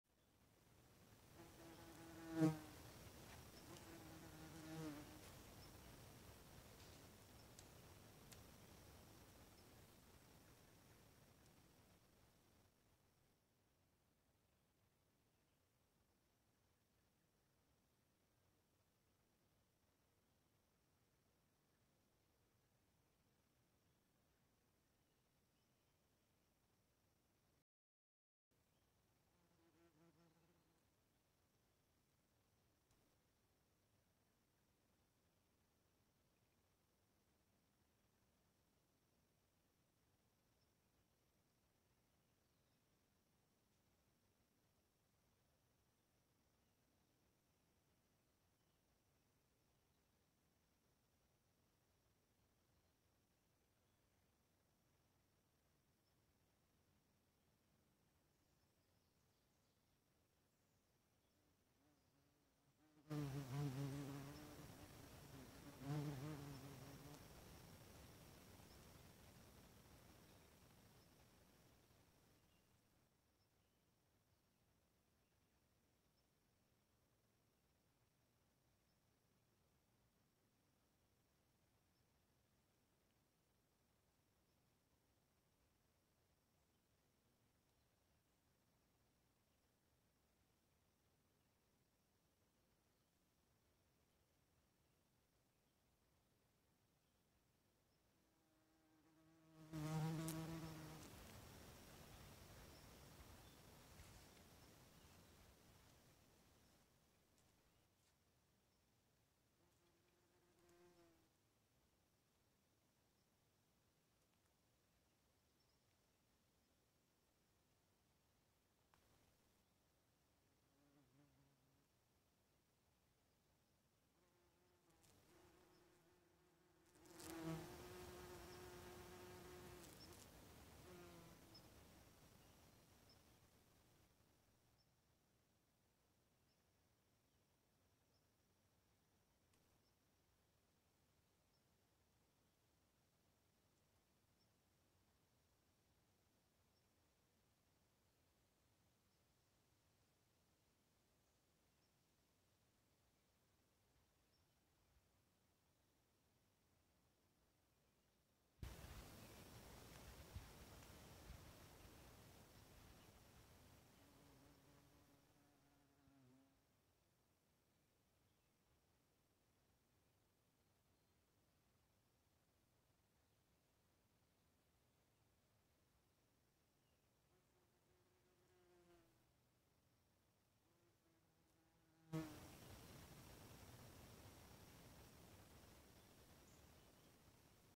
Up-close bald faced hornet nest coming into the evening. No talk, just buzz naturelovers Subscribe
The inevitable sting will do less so with a big subscriber count. Just saying. Bald-faced hornet nest building coming into the evening. No talk, just buzz.